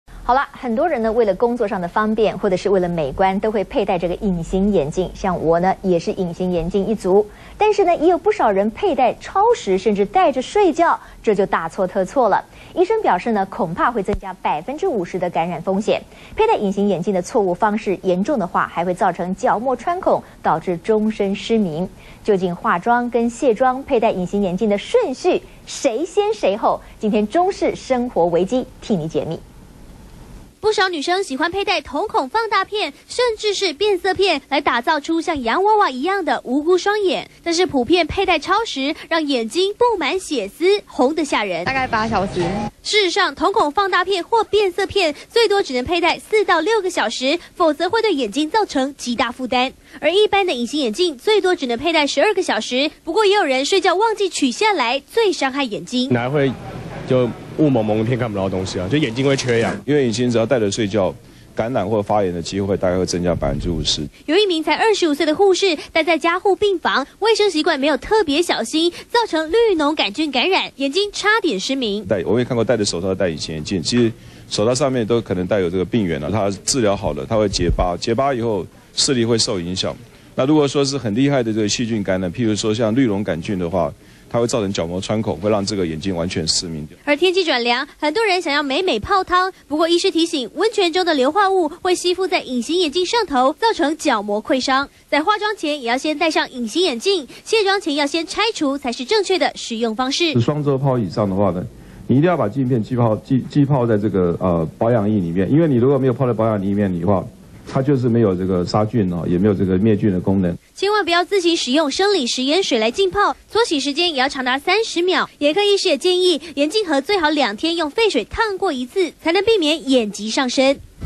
護士戴隱形眼鏡 染綠膿桿菌險瞎
中視新聞》一名25歲的護士在加護病房工作，因加護病房呼吸管等可能藏有綠膿桿菌，每天上班又超過12小時，並配戴隱形眼鏡，沒想到角膜上皮缺損、有傷口，再以手去揉眼睛，導致綠膿桿菌感染，所幸護士第2天立即就醫。